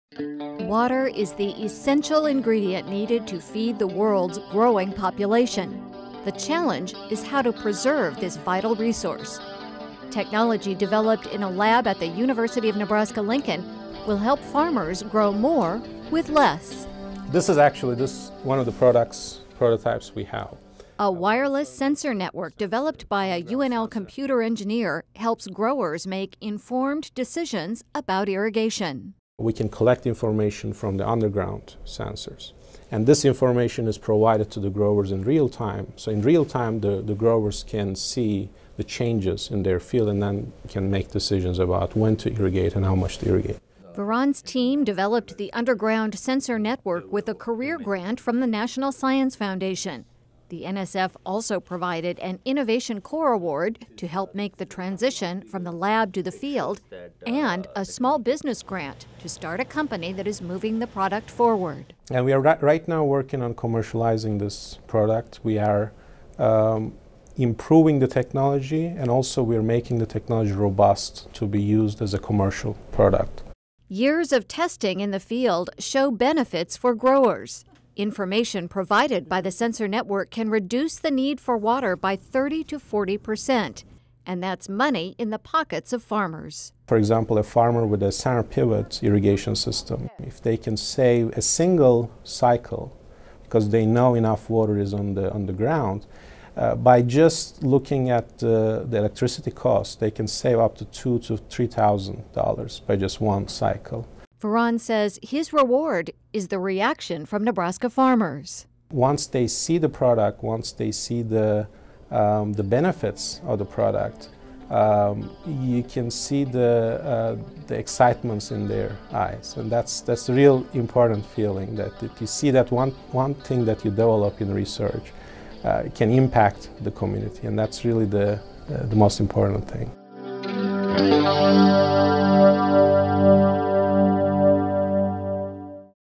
Water-wise Sensor
University of Nebraska-Lincoln computer engineer Mehmet Can Vuran developed technology to help farmers boost yields and conserve water. Now a National Science Foundation initiative to guide promising discoveries toward commercialization is helping him turn his underground wireless sensor network into a practical irrigation management tool.